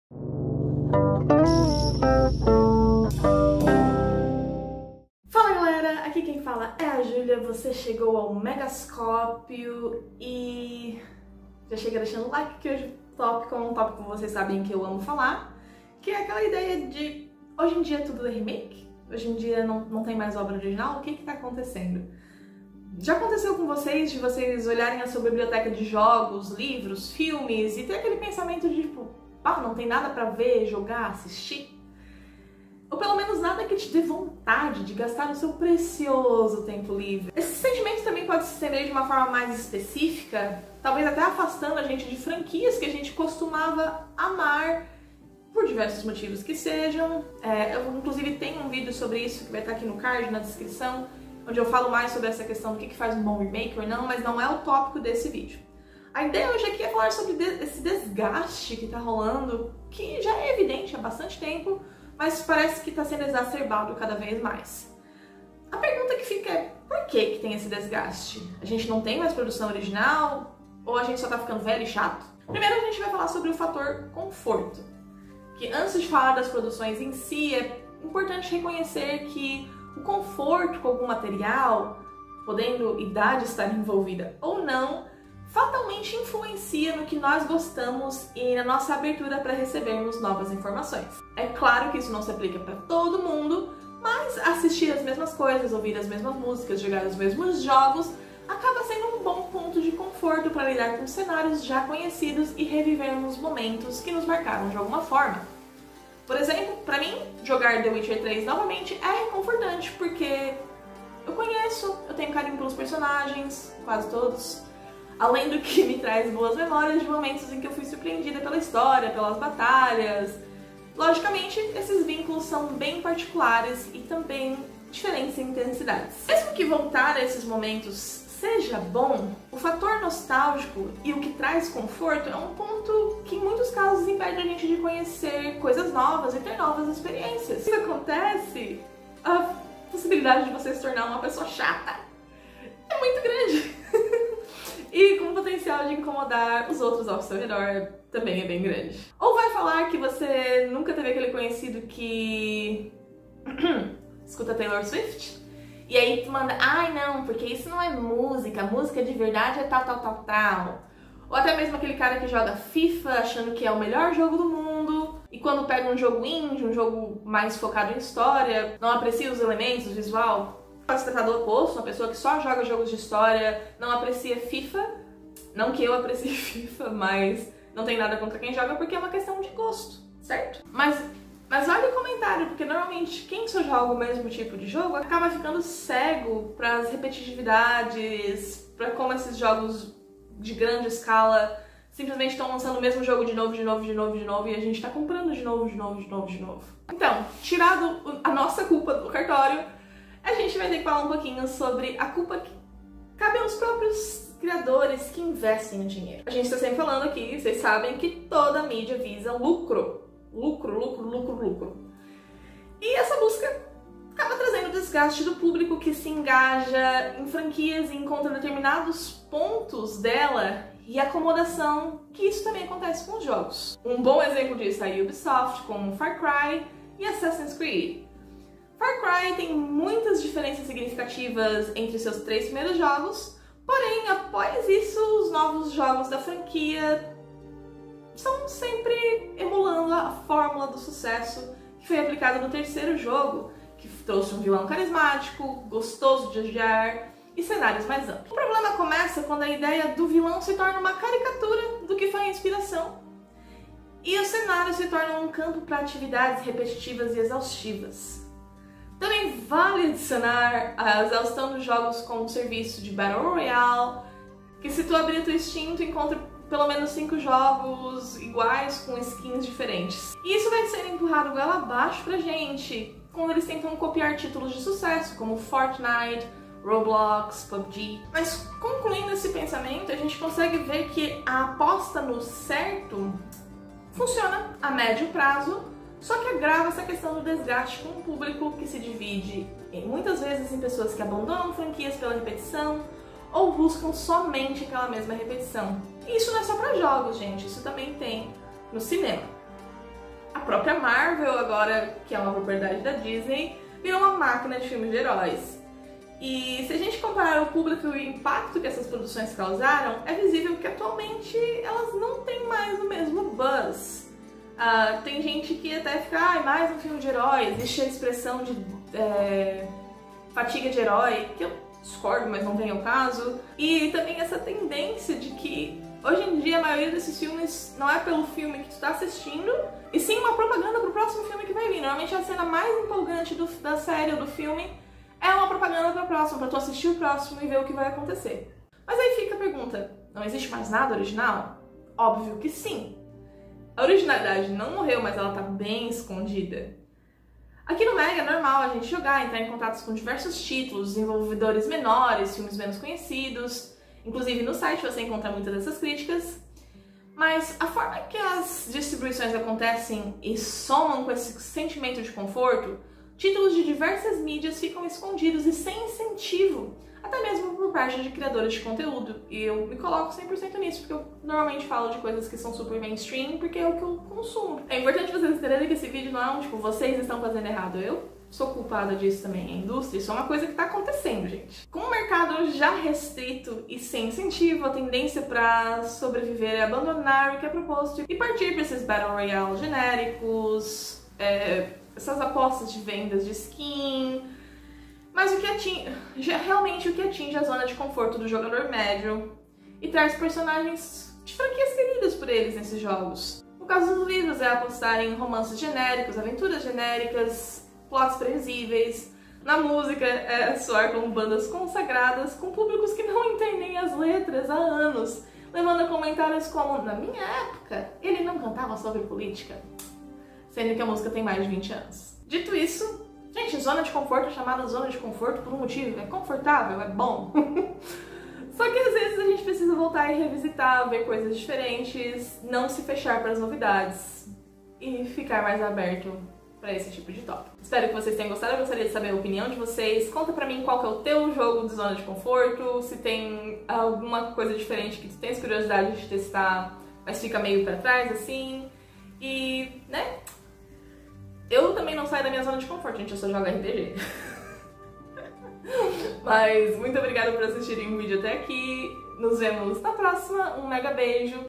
A criatividade MORREU ou está em baixo de uma PEDRA?
Ano após ano remakes, remasters e adaptações são lançadas com diferentes qualidades. Mas e os materiais novos? A criatividade morreu ou está escondida?  Vamos trocar uma ideia e o quais podem ser as causas disso?

ROTEIRO E EDIÇÃO: Ju, Renan